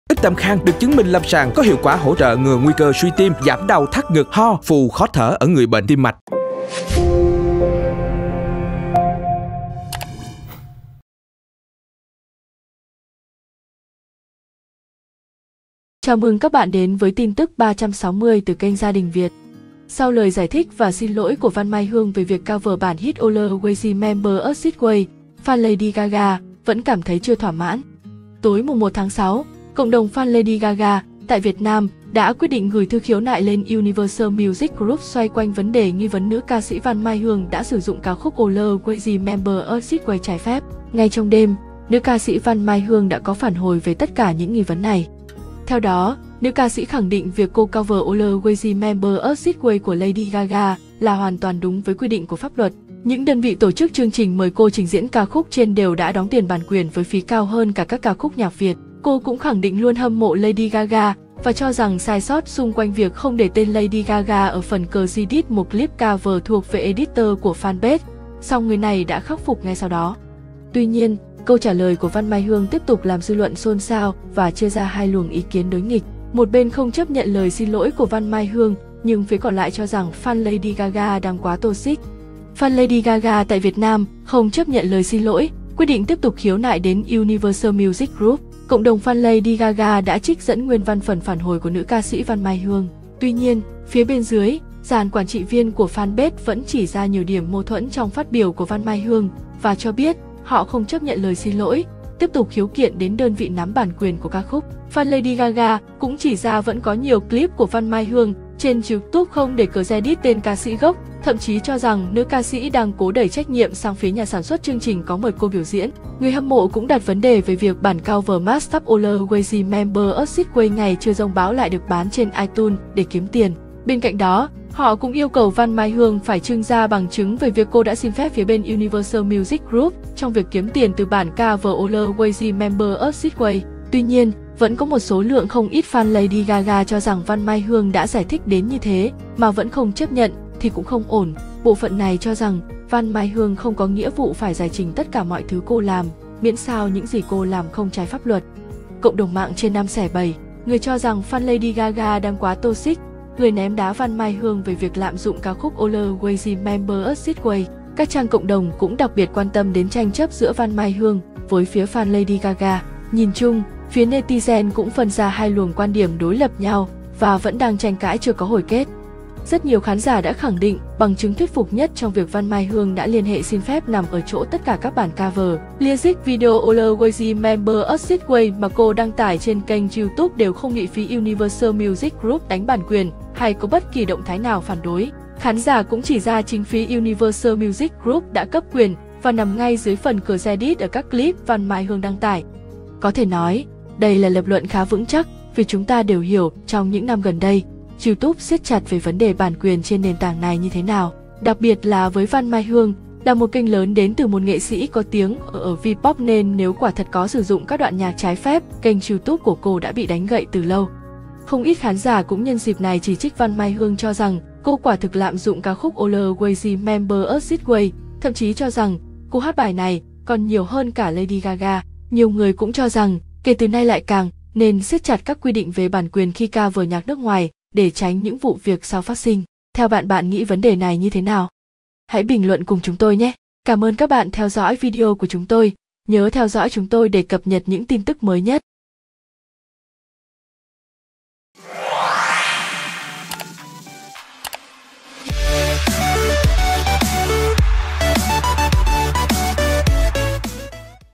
Fan LADY GAGA tuyên bố kiện VĂN MAI HƯƠNG cho đến cùng dù cô đã xin lỗi
Fan LADY GAGA không chấp nhận lời xin lỗi của VĂN MAI HƯƠNG và sẽ kiện cho tới cùng